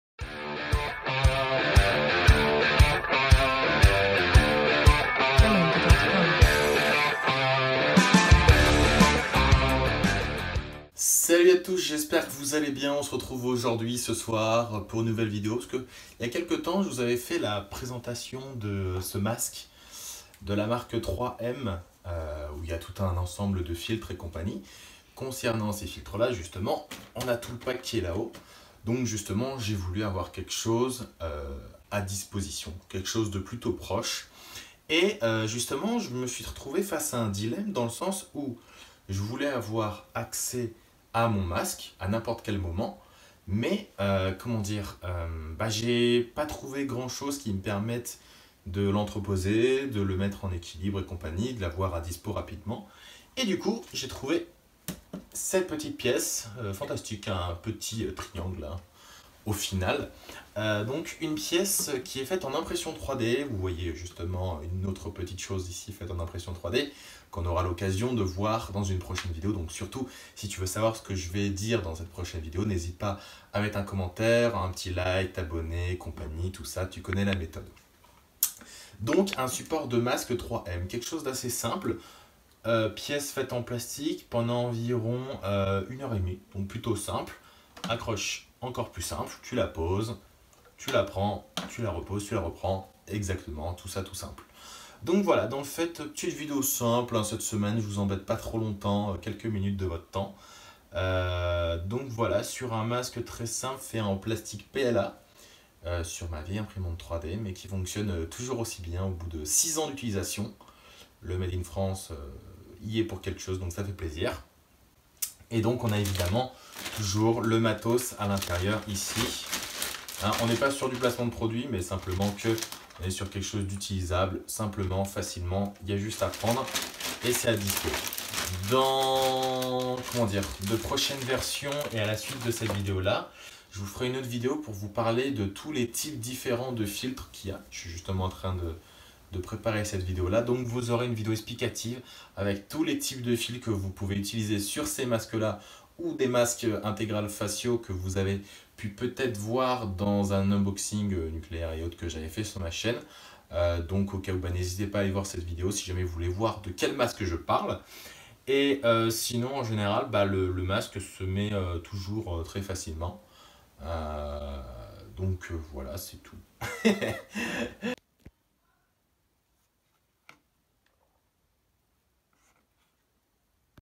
Support Demi Masque 3M
Salut, j'espère que la video te plaira.

Mon Masque 3M Kit Demi-masque 6200 Taille M Serie 6000

0:00 Time lapse impression 3D Support
0:10 Intro
0:58 Support en fonctionnement
1:04 Suite
1:55 Contenu Sac 3M
2:13 Fin
Et mon mail perso pour partenariat, ou questions, suggestion pour mes videos